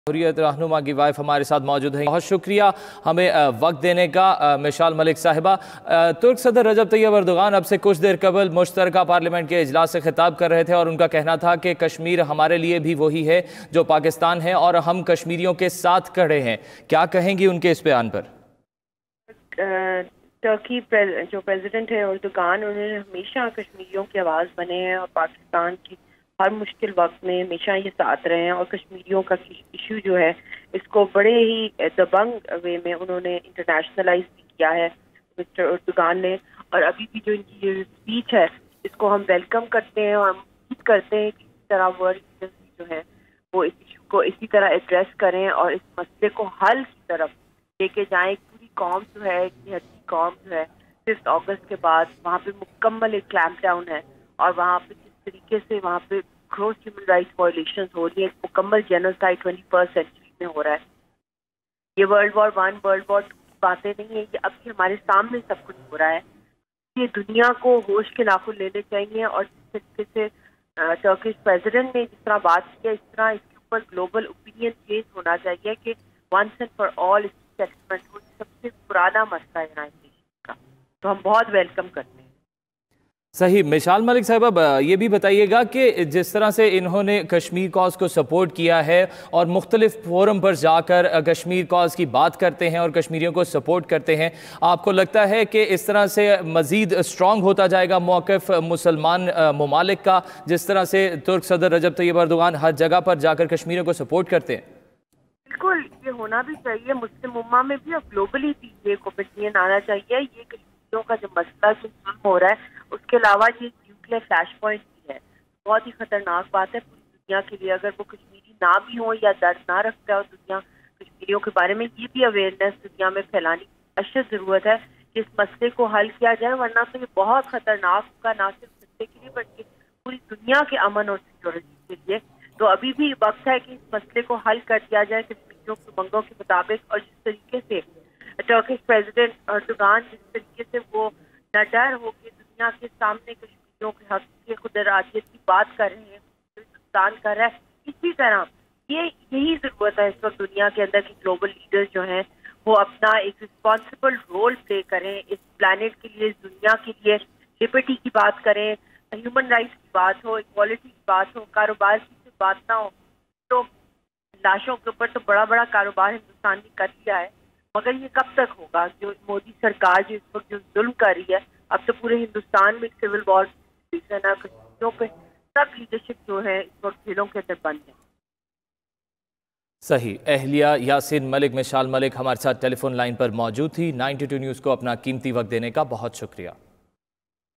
‘Kashmir as important to Turkey as it is to Pakistan’: Tayyip Erdogan | Mashaal Malik appreciates
Mashaal Malik appreciates historical statement of Turkish President | 14 February 2020 | 92NewsHD